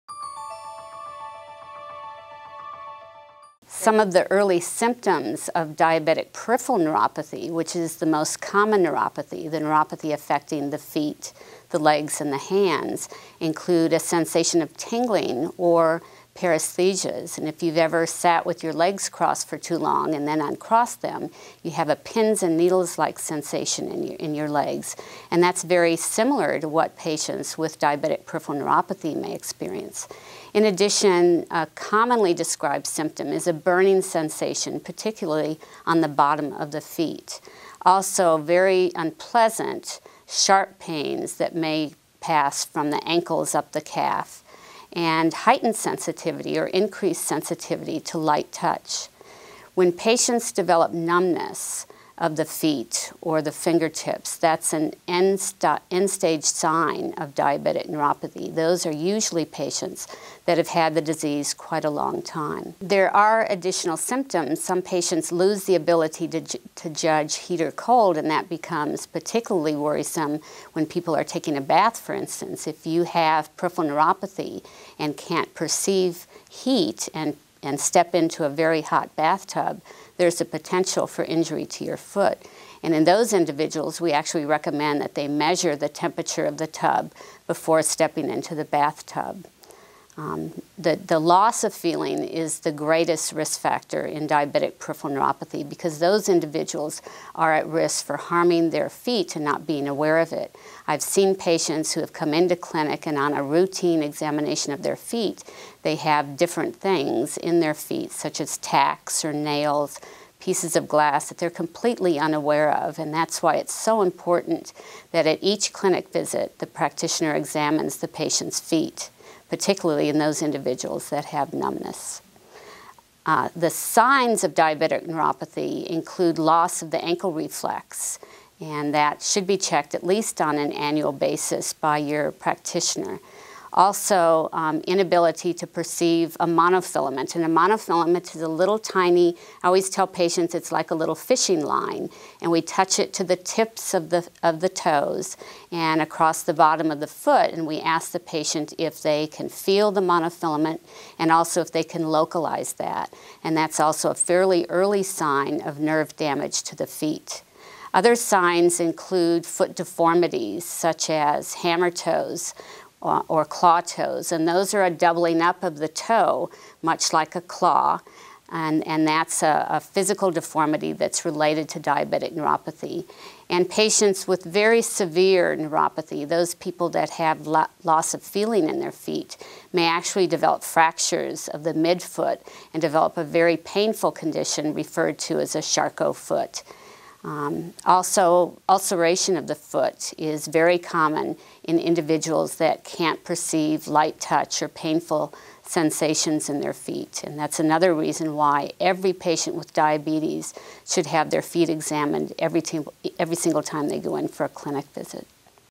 What Are Symptoms of Diabetic Nerve Damage?
Learn about diabetic nerve damage. MDs answer your diabetes questions.